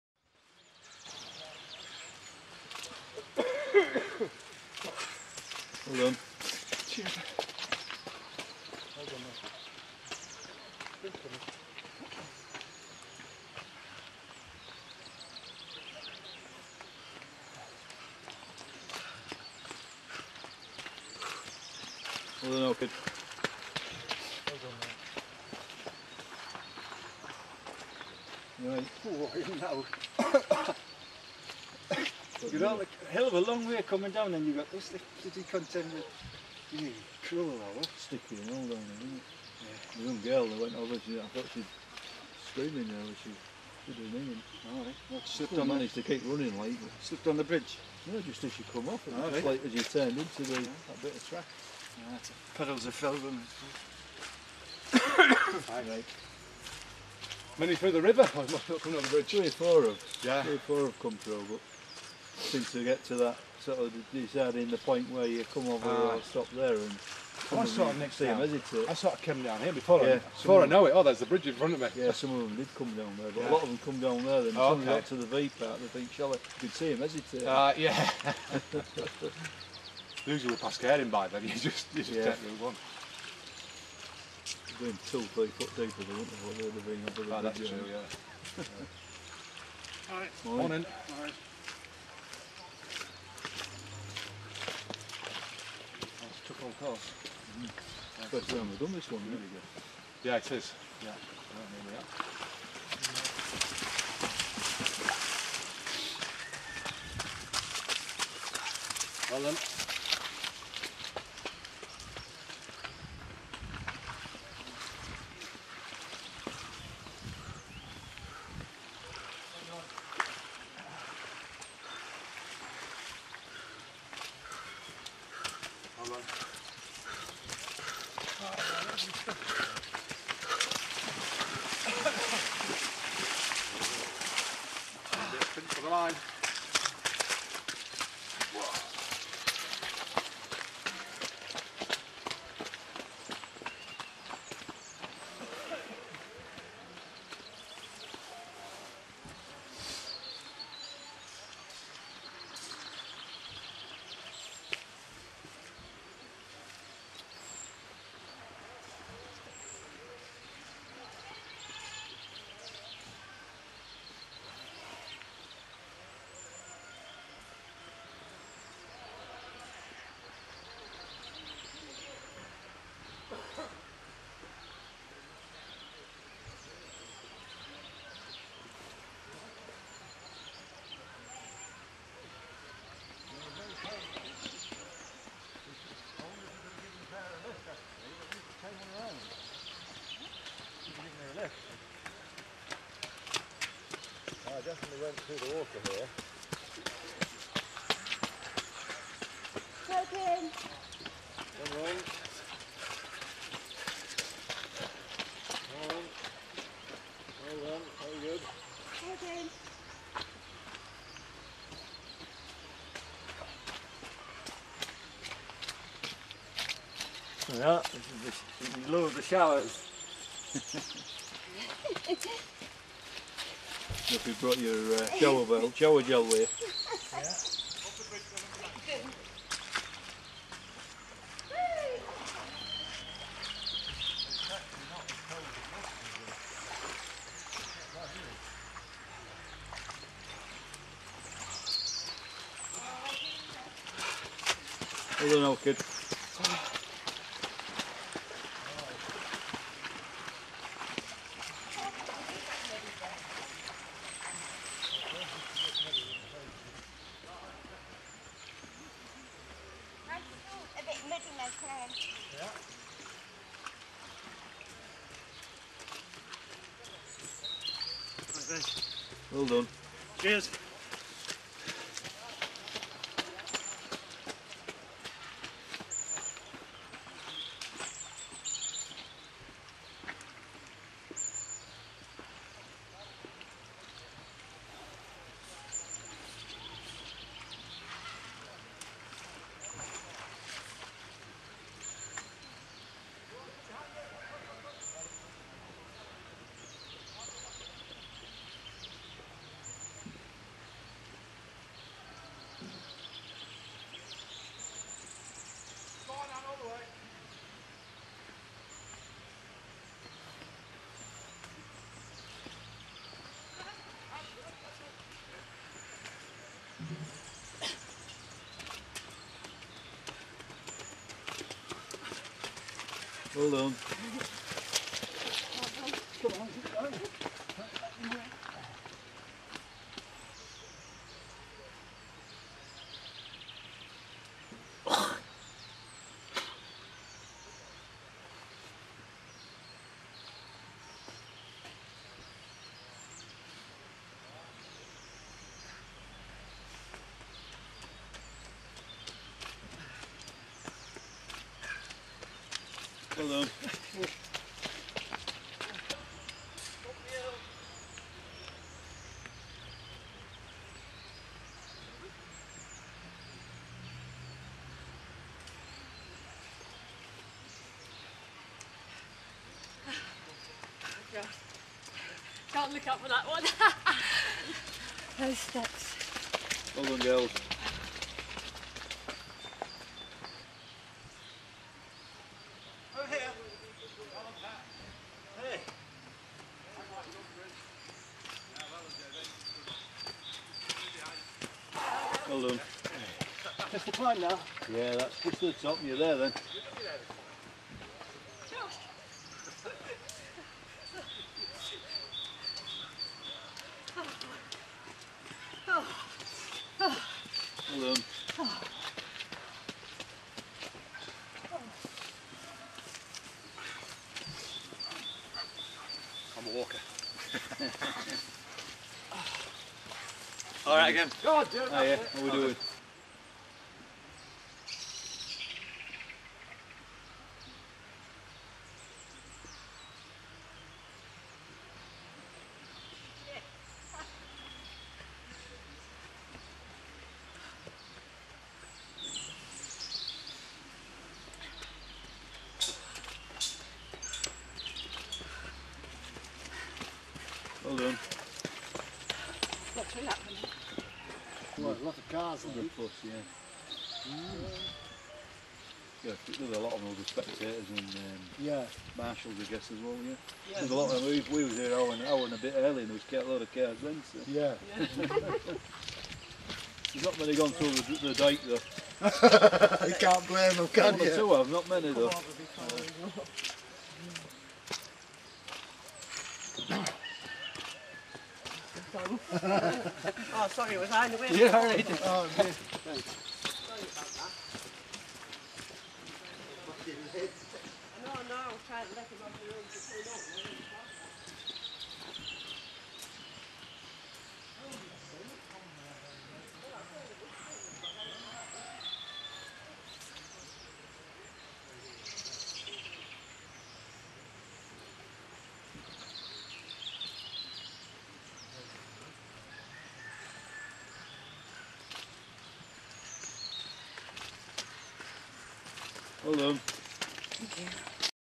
Glaisdale Rigg - Fell Race - 2011 PART D
The In-original - Glaisdale Rigg Fell Race - From the  Amecliffe Arms, Glaisdale, Guisborough North Yorkshire 13th March 2011